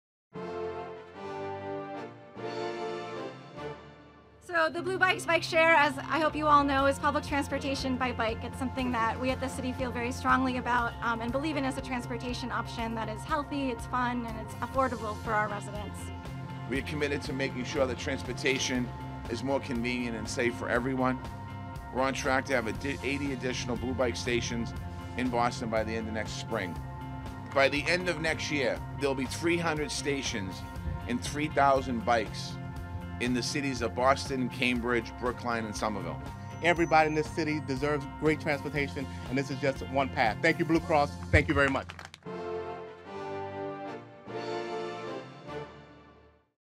Bluebikes Celebration in Mattapan - Promo
Mayor Walsh joins community leaders to celebrate the expansion of Blue Bikes to Mattapan. This program is an affordable, healthy and fun form of public transport, and will now be available to even more people in Boston.